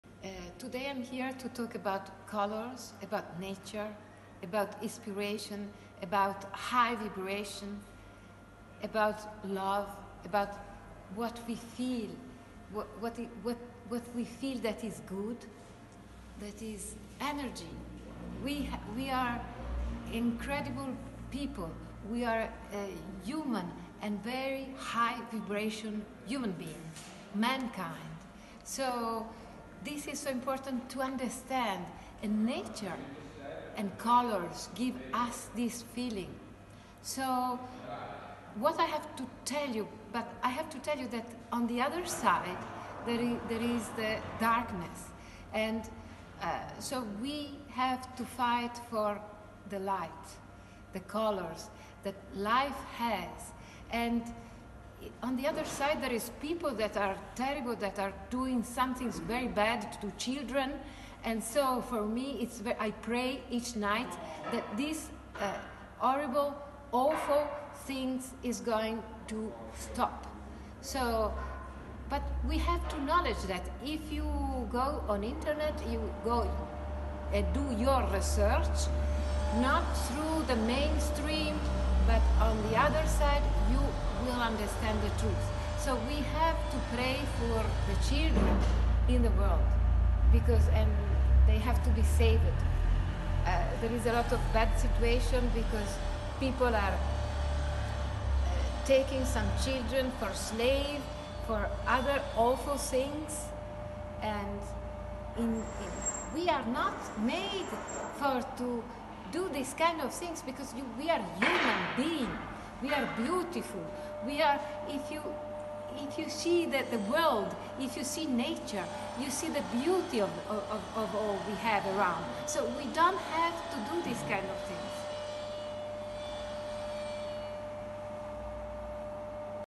Colours in action
This video was made by the video maker Paul Goglien , in 2020, Brodaska was inspired by Timothy Ballard , and his organisation which is saving many children around the world from pedophilia, and of consequence today of Sound of freedom, the Movie of year of Jim Caviezel